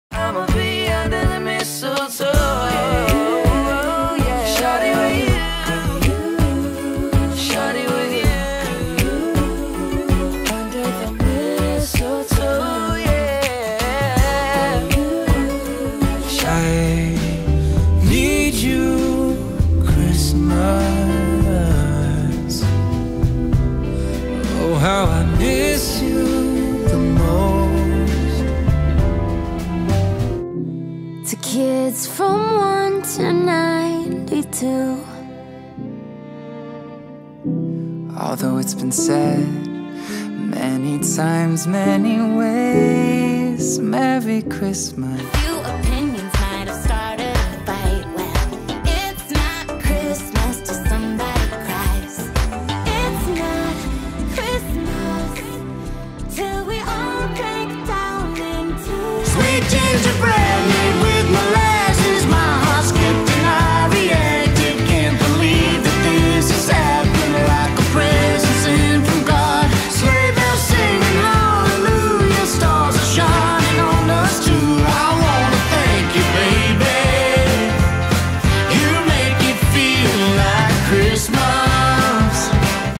5 músicas natalinas para entrar no clima!
É incrível quando os artistas que amamos surgem com conteúdos especiais para o fim de ano. Confira 5 músicas natalinas para entrar no clima do Natal: